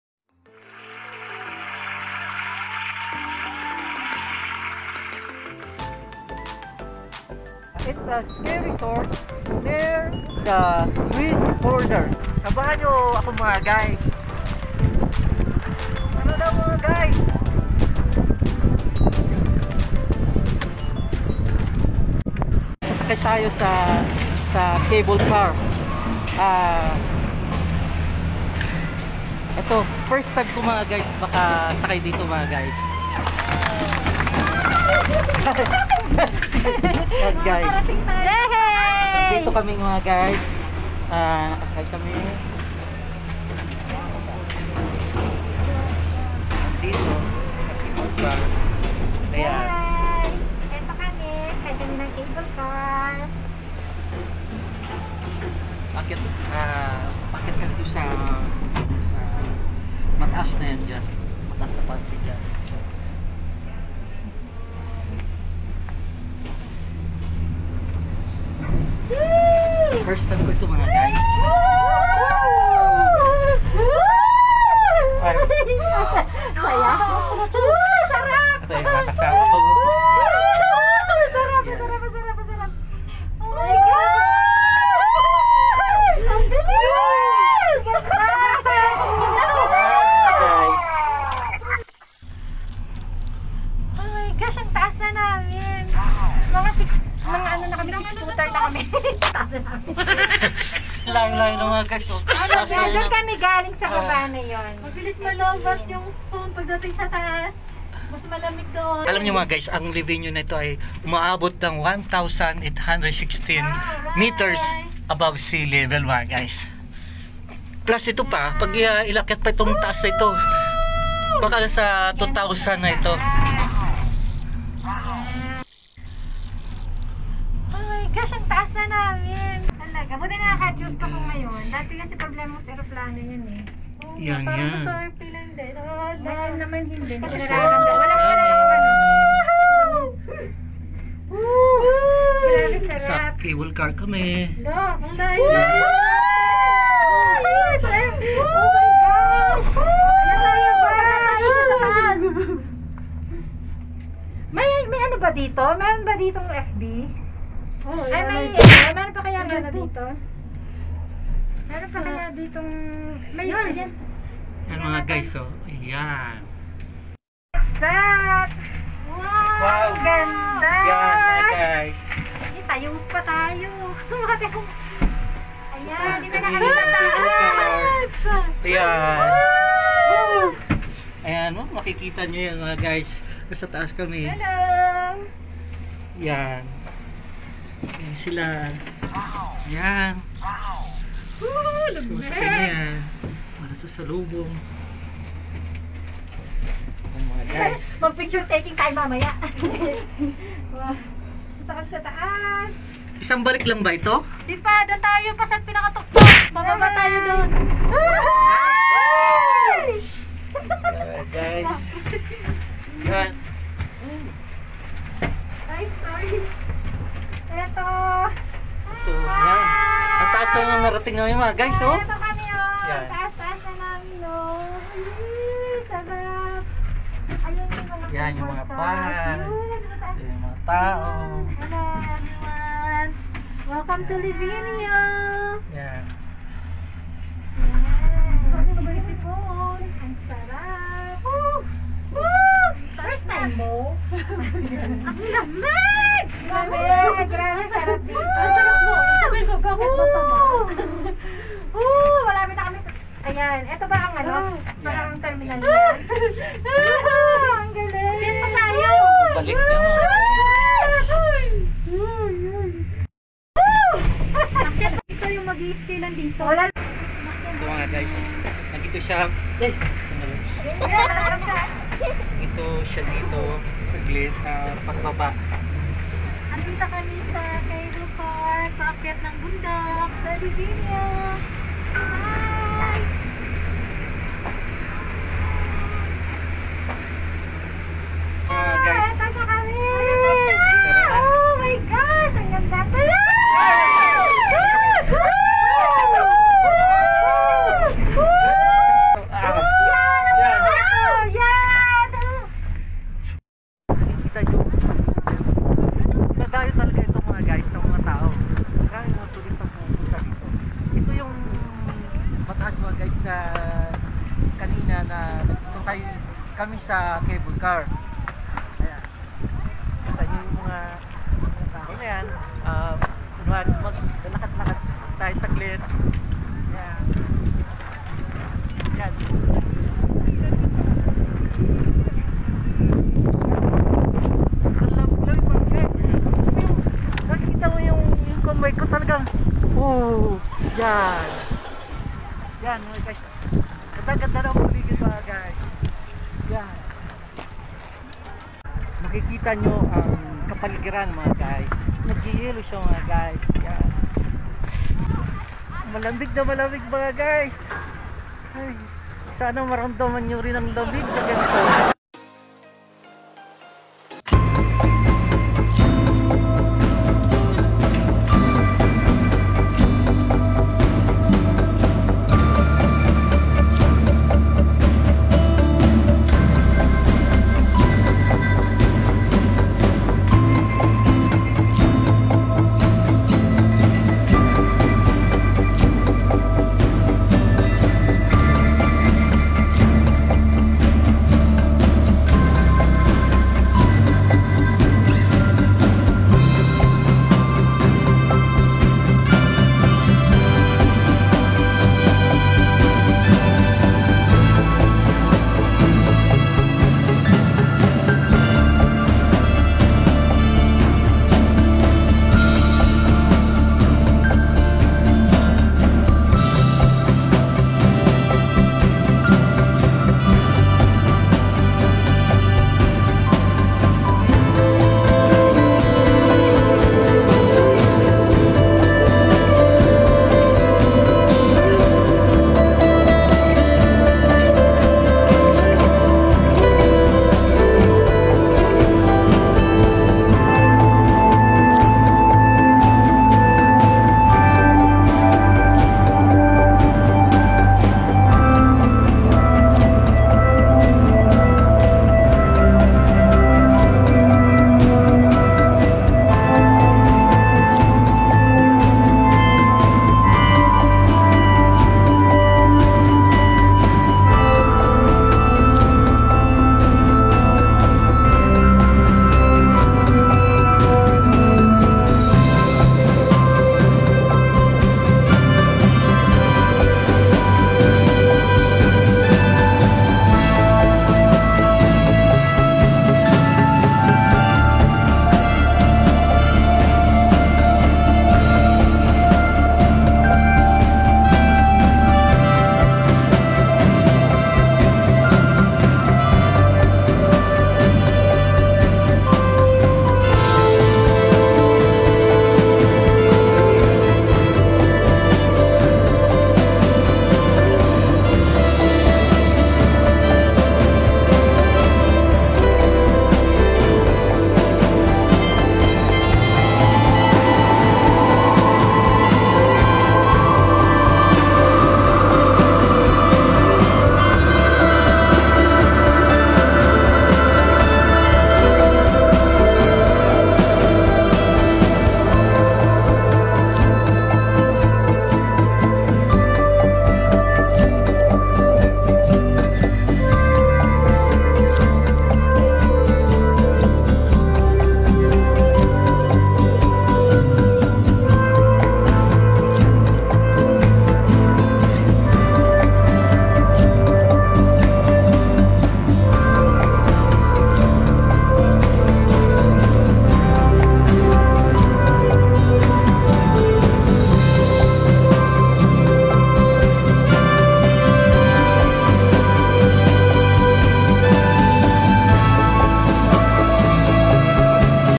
First Time Makasakay ng Cable Car | Exciting Experience
Livigno is a ski resort in the Italian Alps, near the Swiss border. It represents a premium skiing experience for its visitors. Snowpark and its long, wide and gentle slopes allowing skiers to enjoy the fresh snow, it’s no wonder that Livigno has become such a popular destination and a snowboarders’ paradise!

Close your eyes, take a deep breath, then open your eyes again and head off for some fun in the open air. Welcome to Livigno, the kingdom of outdoor activities, located in the very heart of the Italian Alps.